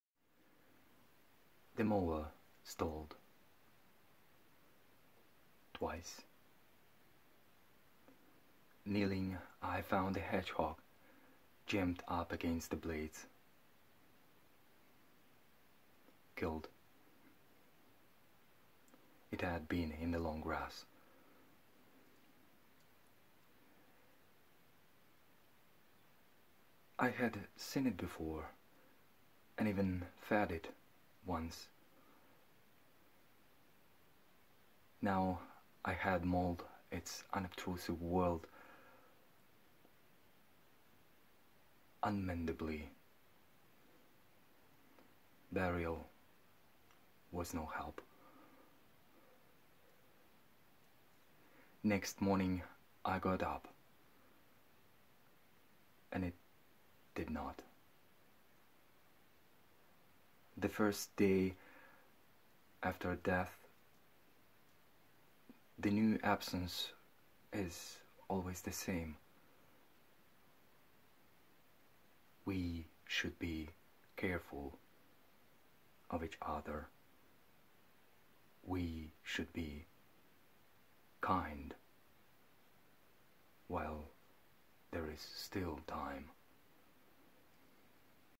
Philip Larkin — The mower (1979)
The mower stalled, twice; kneeling, I found
A hedgehog jammed up against the blades,
Killed. It had been in the long grass.

I had seen it before, and even fed it, once.
Now I had mauled its unobtrusive world
Unmendably. Burial was no help:

Next morning I got up and it did not.
The first day after a death, the new absence
Is always the same; we should be careful

Of each other, we should be kind
While there is still time.